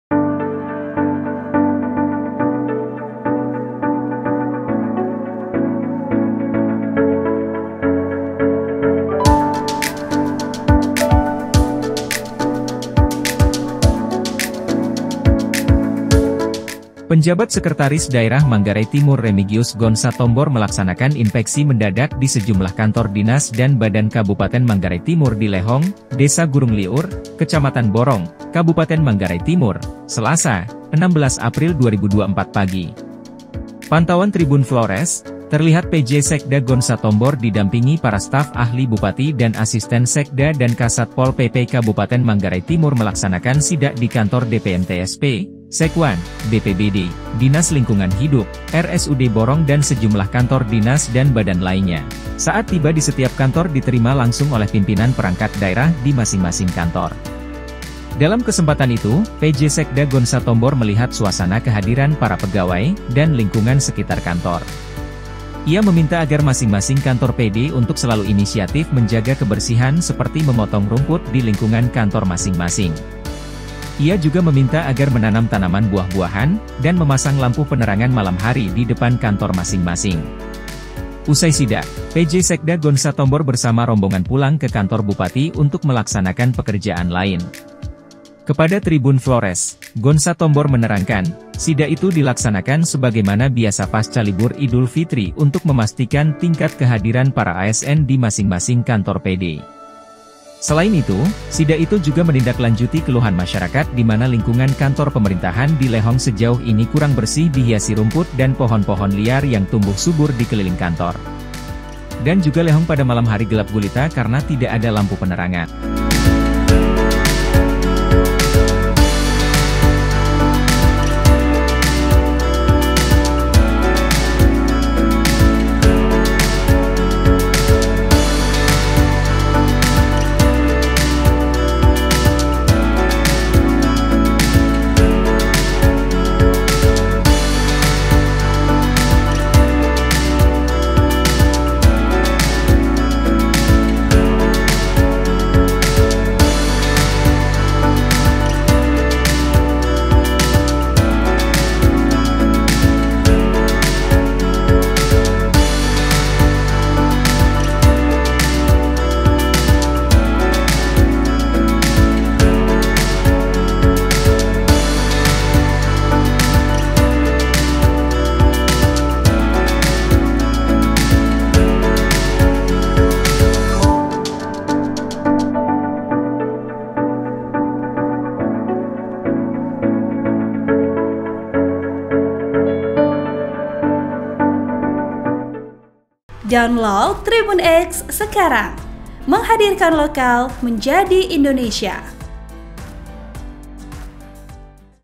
Penjabat Sekda Manggarai Timur Inpeksi Mendadak di Kantor Perangkat Daerah
TRIBUNFLORES.COM, BORONG - Penjabat Sekertaris Daerah (Sekda) Manggarai Timur Drs. Remigius Gonsa Tombor melaksanakan inpeksi mendadak (sidak) di sejumlah kantor dinas dan badan Kabupaten Manggarai Timur di Lehong, Desa Gurung Lieur, Kecamatan Borong, Selasa 16 April 2024 pagi.

Pantauan POS-KUPANG.COM, terlihat Penjabat Sekda Gonsa Tombor didampingi para staf ahli bupati dan asisten sekda dan Kasat Pol PP Kabupaten Manggarai Timur melaksanakan sidak di Kantor DPMTSP, Sekwan, BPBD, Dinas Lingkungan Hidup, RSUD Borong dan sejumlah kantor dinas dan badan lainya.

Ia meminta agar masing-masing kantor PD untuk selalu inisiatif menjaga kebersihan seperti memotong rumput di lingkungan kantor masing-masing.

Artikel ini telah tayang di Pos-Kupang.com dengan judul Penjabat Sekda Manggarai Timur Inpeksi Mendadak di Kantor Perangkat Daerah,

Penulis: Robert Ropo
Editor: Eflin Rote
VP: Ian Taopan